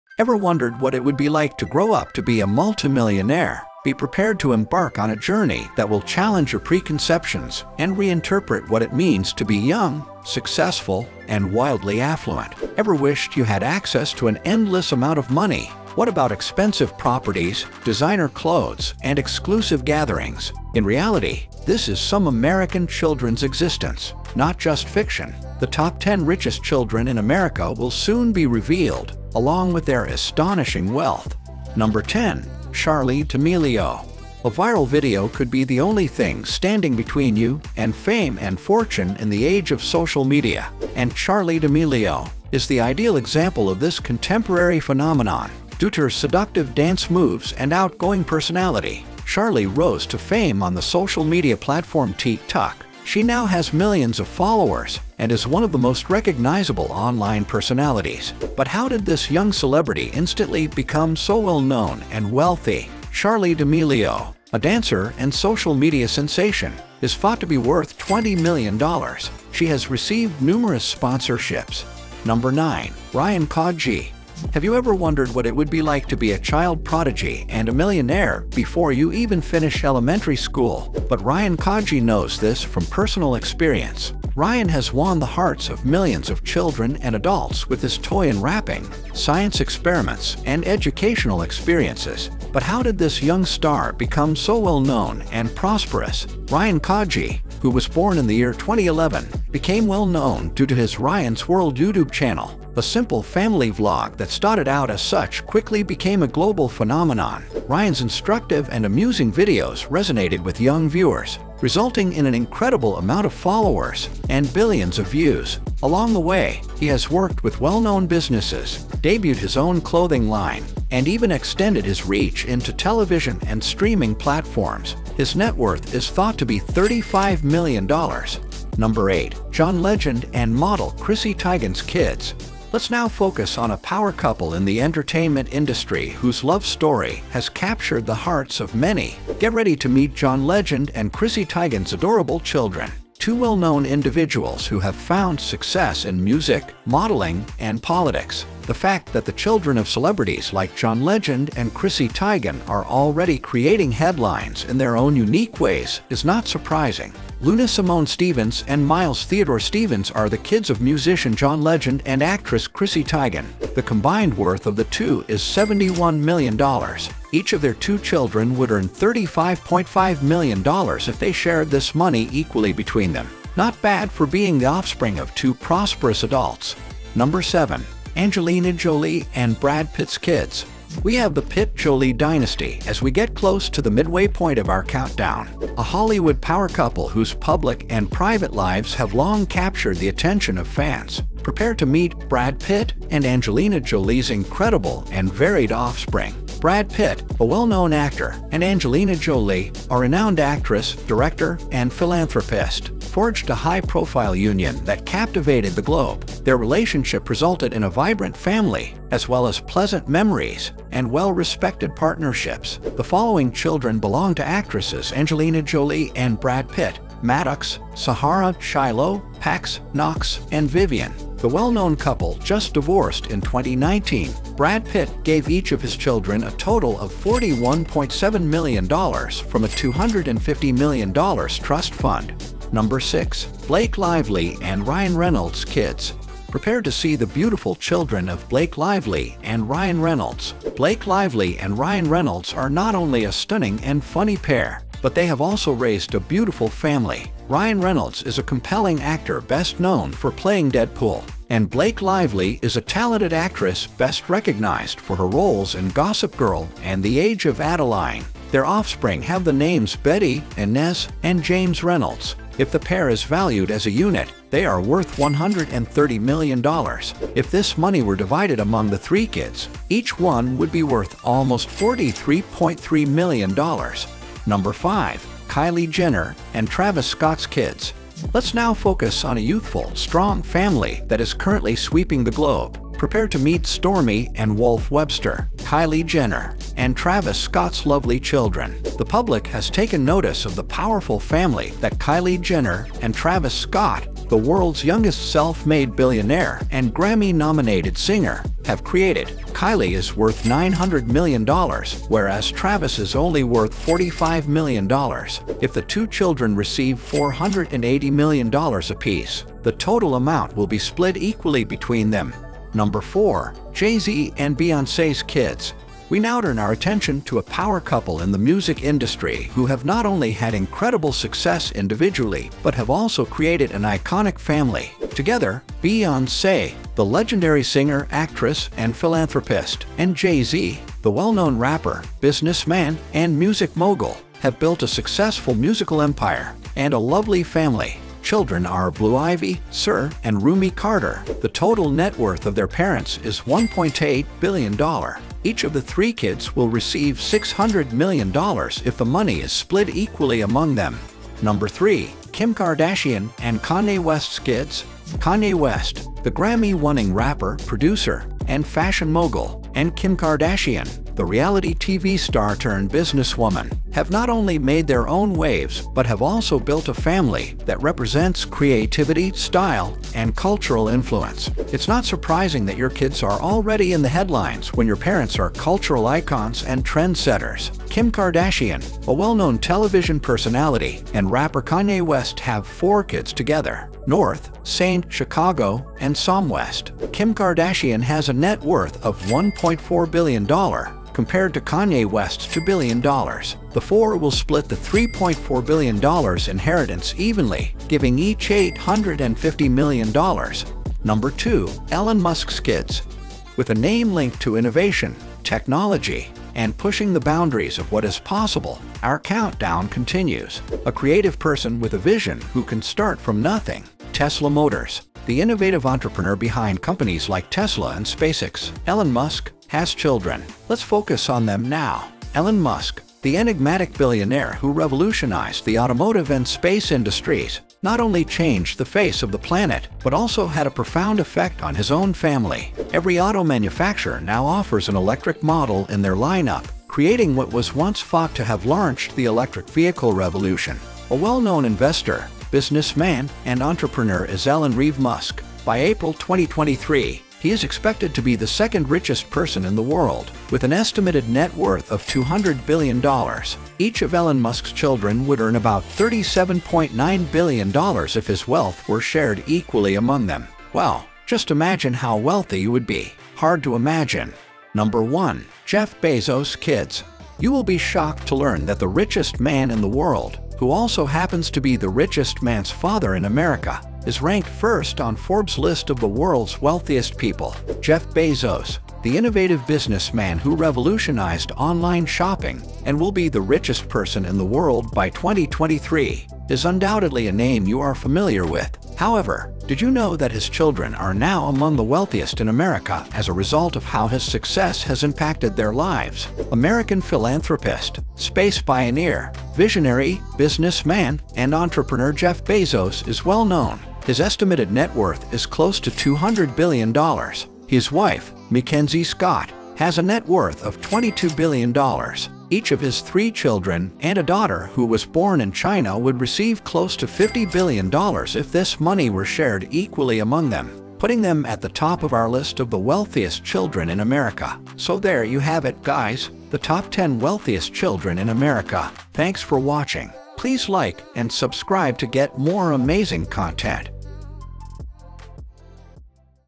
The Richest Kids Who Own America
America is the richest country in the world and it has some richest children. In today's video we will introduce you the richest kids who owns the America. These top 10 richest kids in the US who make the money from his family or YouTube, TikTok. Inside the richest lives of richest kids in USA will make you feel poor. Who is the richest kid in America? Their net worth, luxury lifestyle envious you. So let's start today's video richest kids in USA 2023.